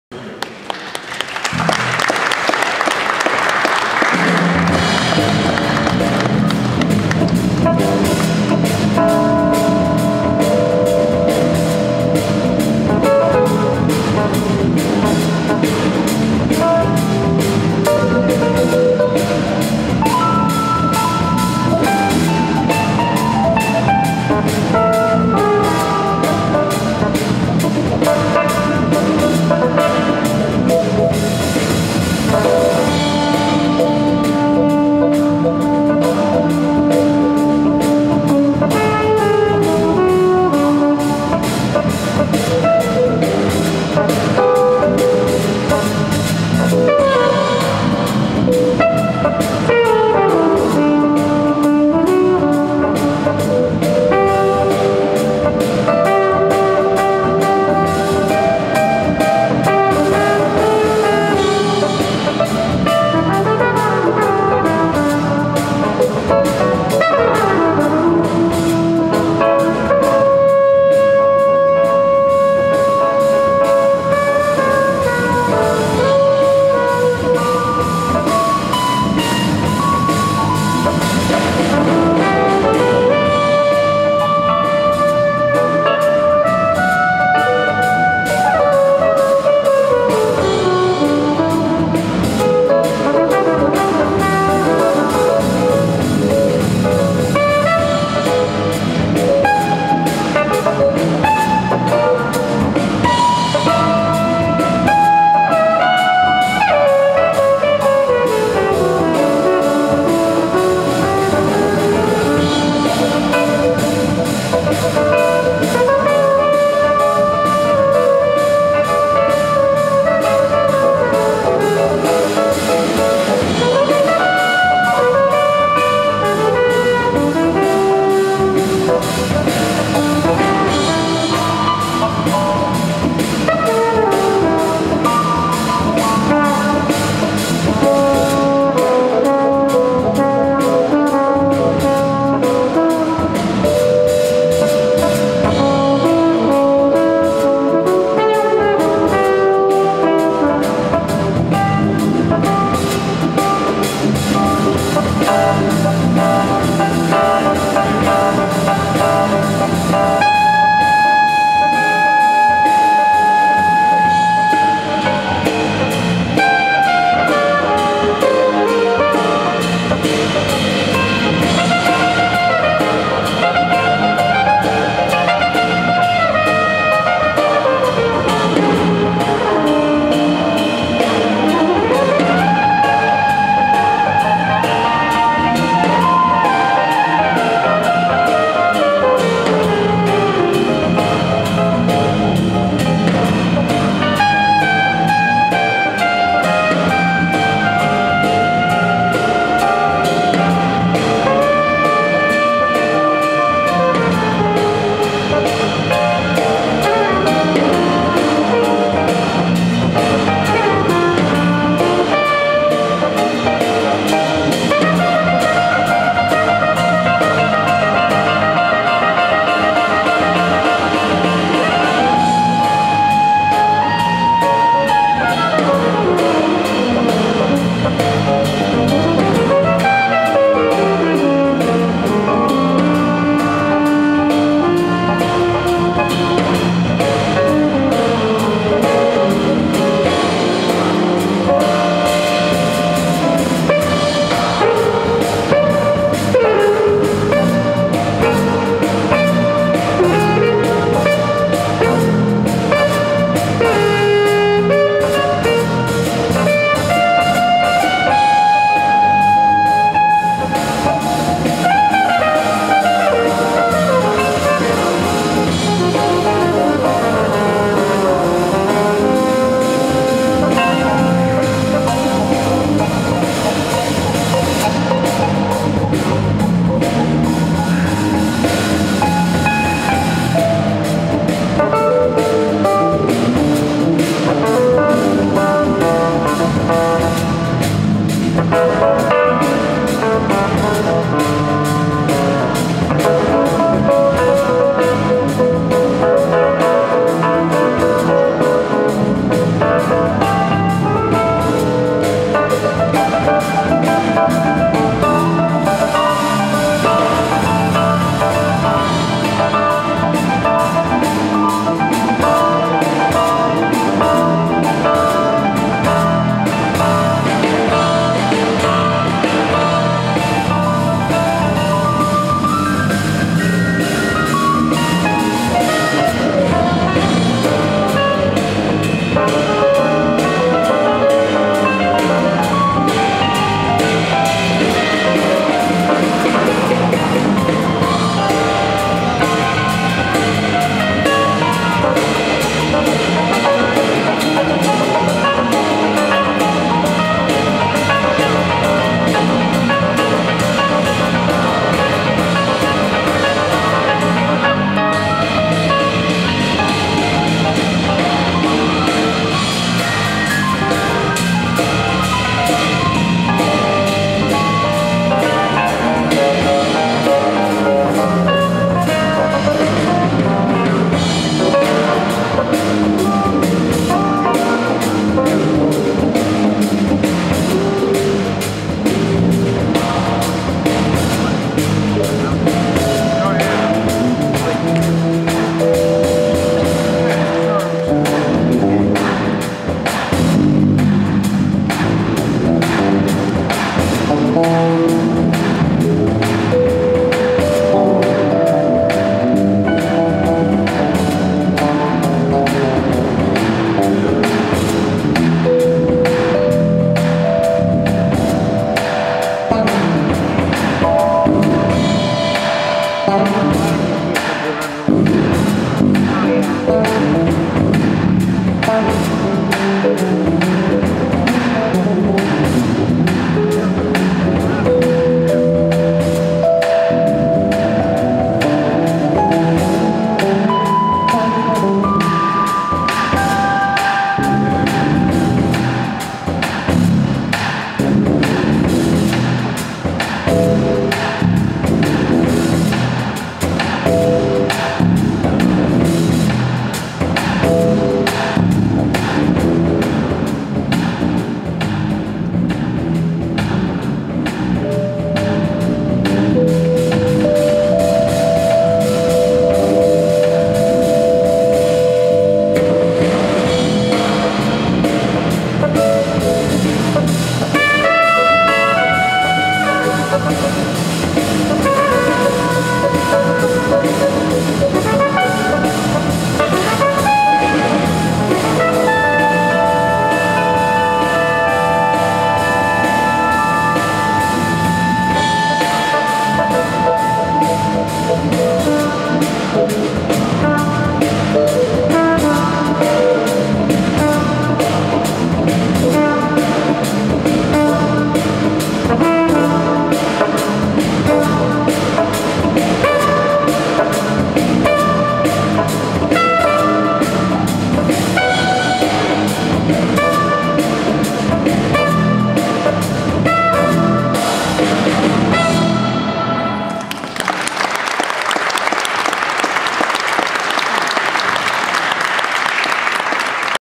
Kenny Davis Jazz Quartet - concert excerpt
Performed during May 11, 2016 Brownbag Concert at Trinity Cathedral (Episcopal), Cleveland, Ohio.
Kenny Davis Jazz Quartet
Kenny Davis, trumpet
Jackie Warren, piano
Kurt Felgemaker, bass
Ron Godale, drums
Moises Borges, guitar/vocals